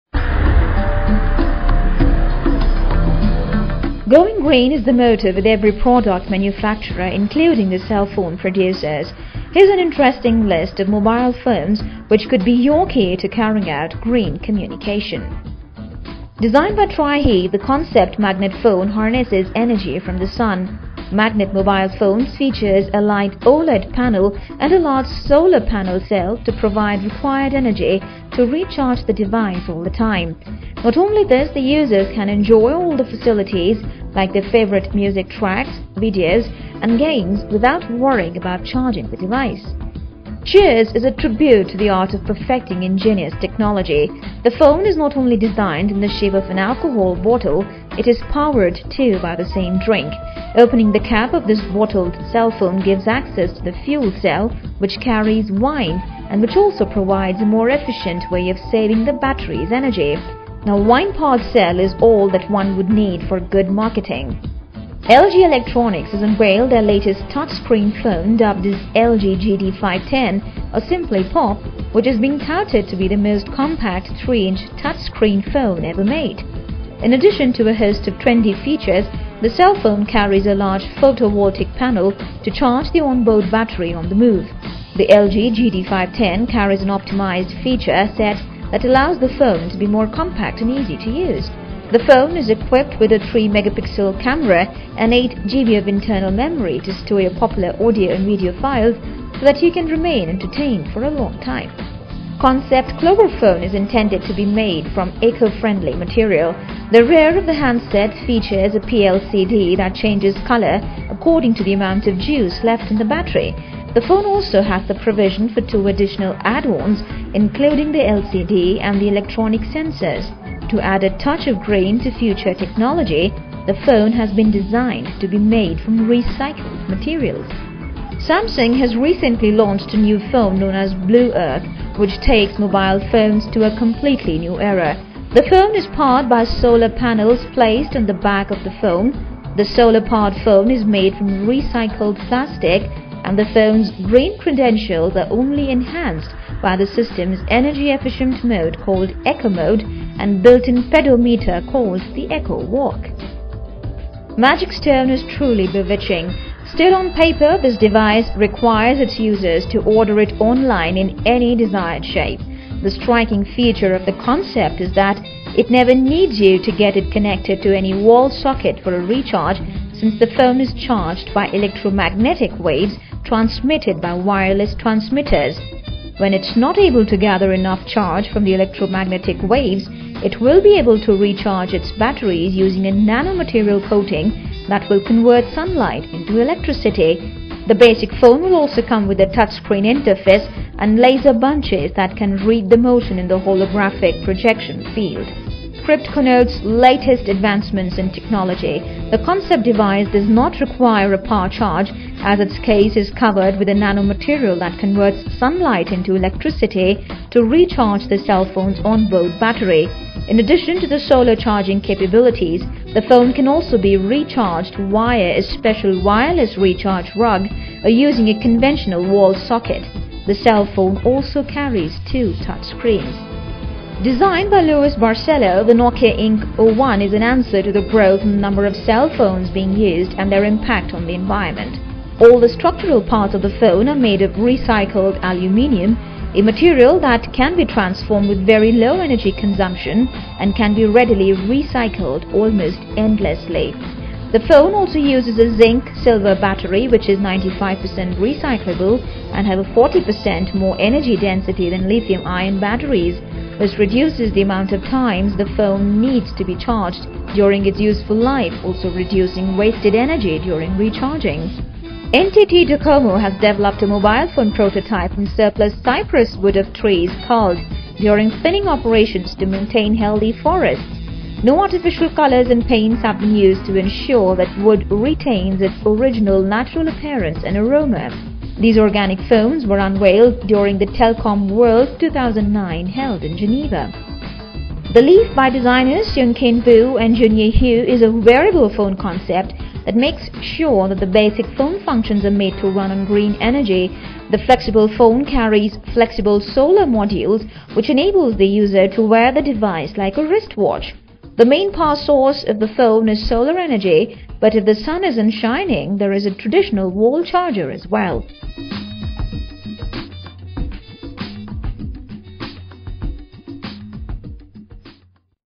TOP GREEN MOBILE PHONES OF 2009
Going green is the motto with every product manufacturer including the cell phone producers. Heres an interesting list of mobile phones, your key to carrying out green communication.